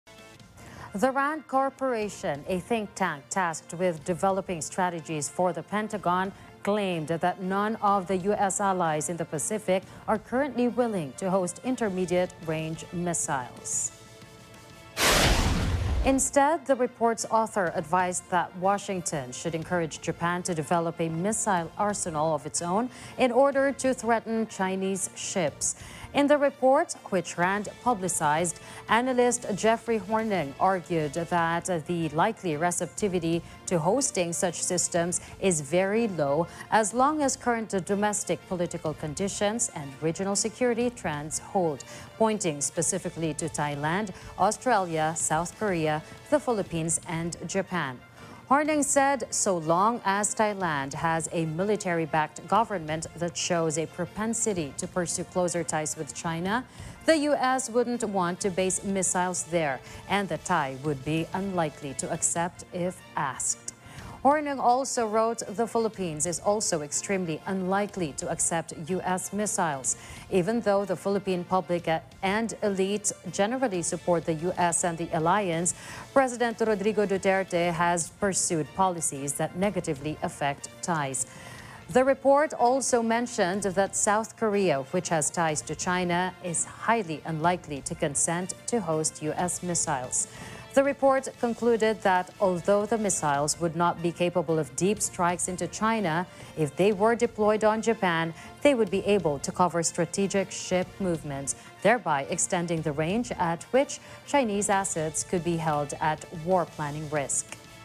New US strategy seeks to arm Japan vs. China
The Rand Corporation, a think-tank tasked with developing strategies for the Pentagon, claimed that none of the US allies in the pacific are currently willing to host intermediate-range missiles.